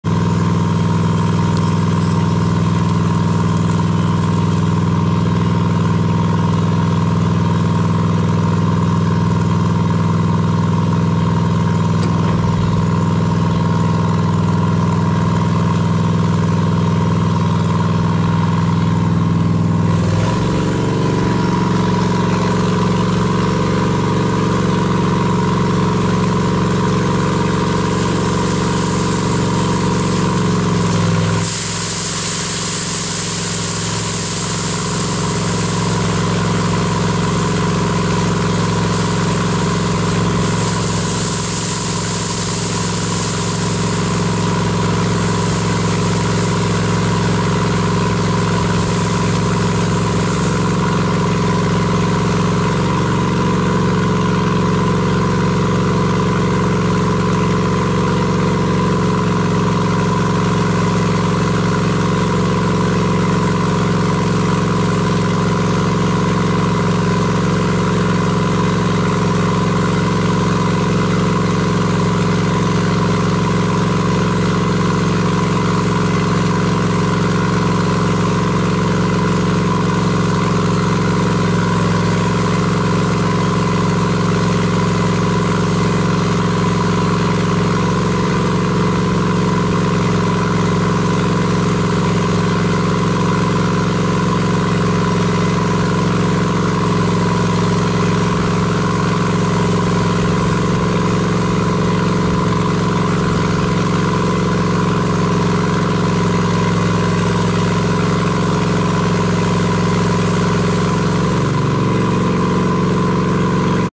novacool at 0.5% class B fire
Class B fire 3gals. E-10 gas, 3gals. diesel.